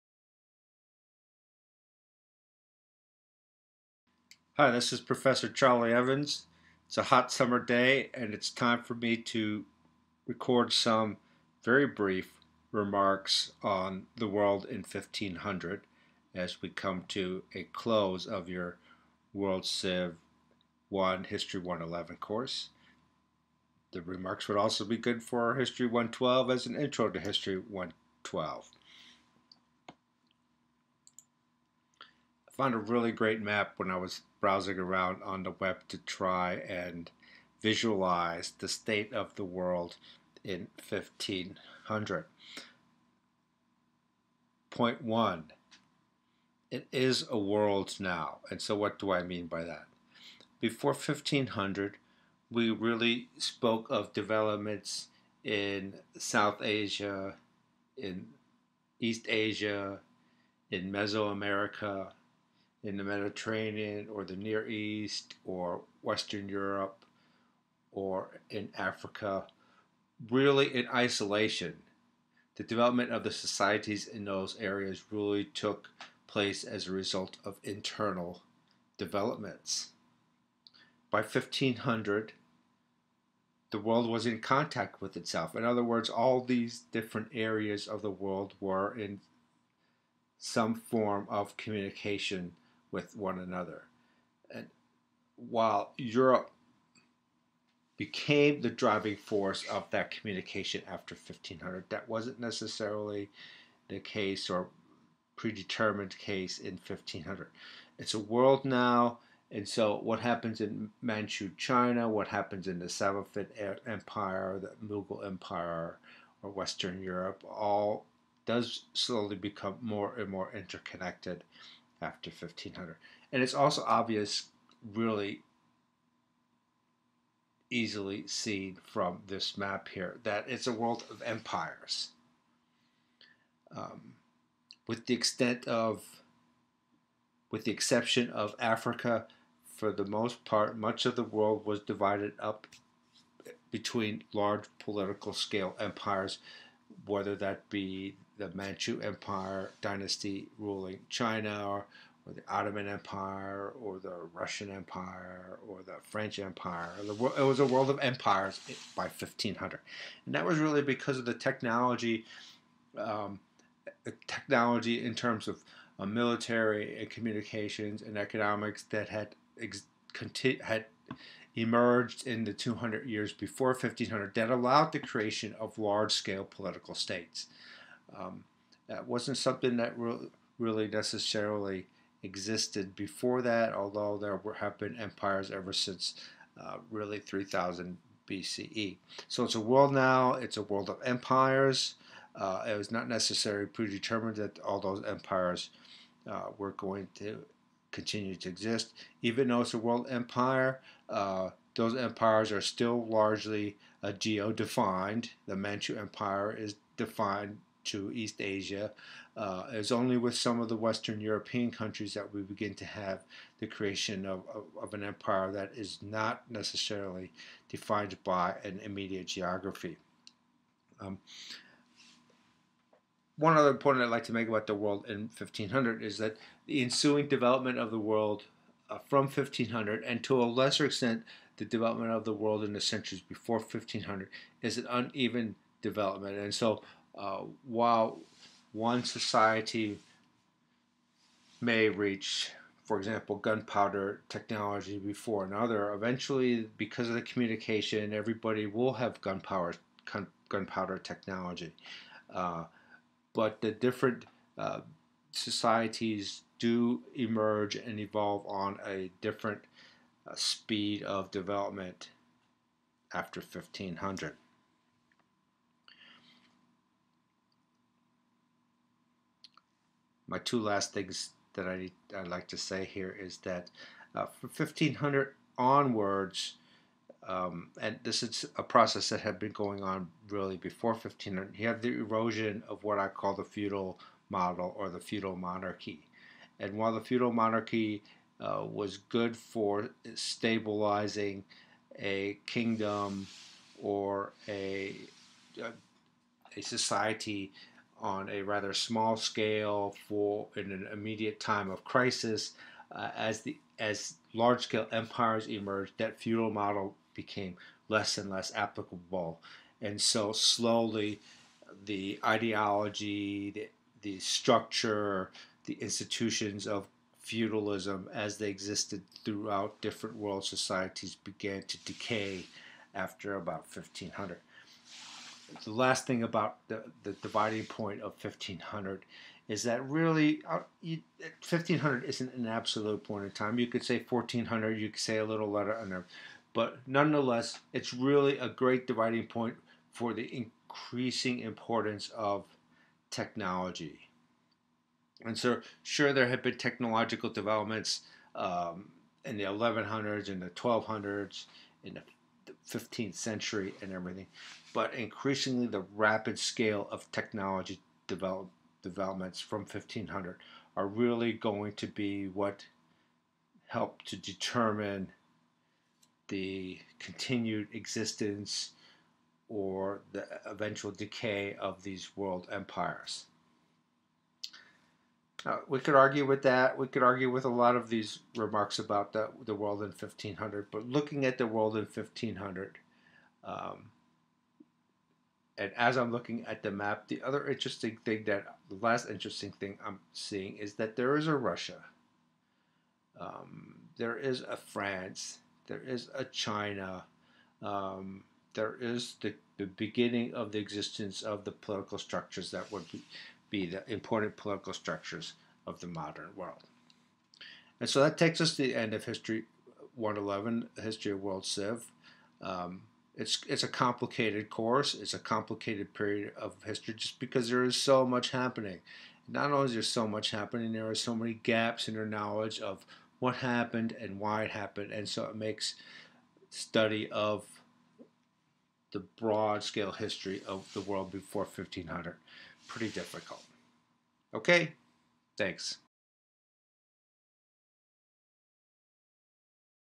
World in 1500: Some Real Quick Remarks
A very short video on the world in 1500 for our online world civilization survey history course.